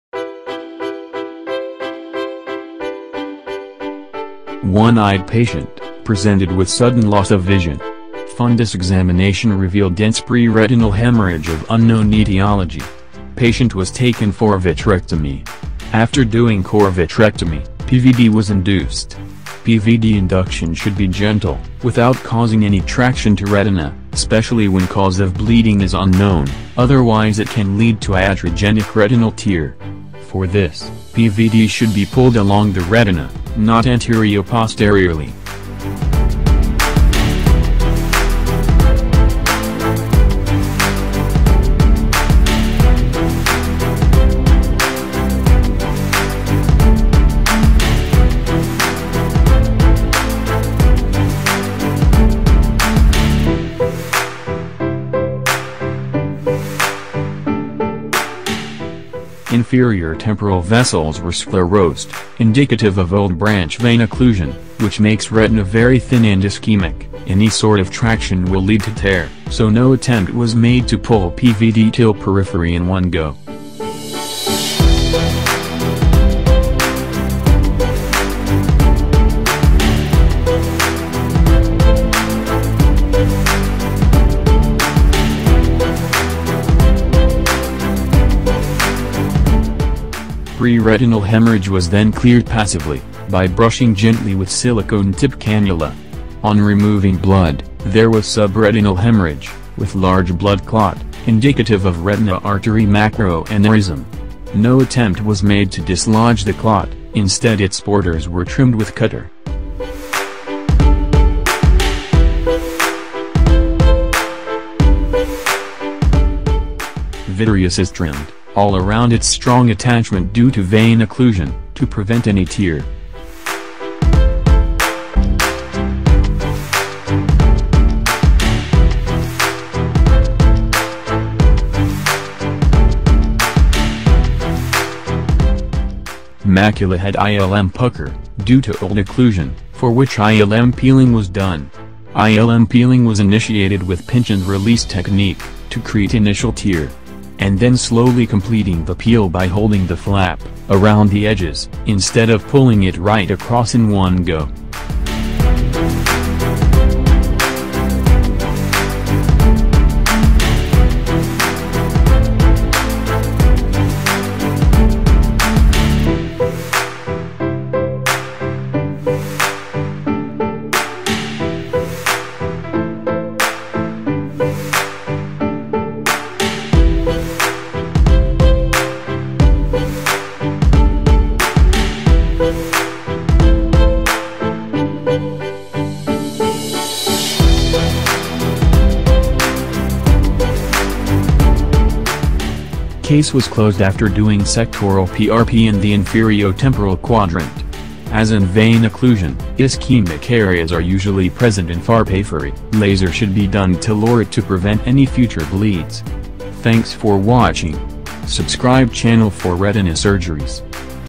RAM: RETINAL ARTERY MACROANEURYSM
RAM CAN CAUSE MASSIVE BLEEDING UNDER THE RETINA, WITHIN LAYERS OR PRE RETINAL SPACE, CAUSING SUDDEN DROP IN VISION. THIS VIDEO I AM POSTING TO DEMONSTRATE THE TECHNIQUE OF HANDLING SUCH CASES SURGICALLY IN MOST EFFECTIVE WAY.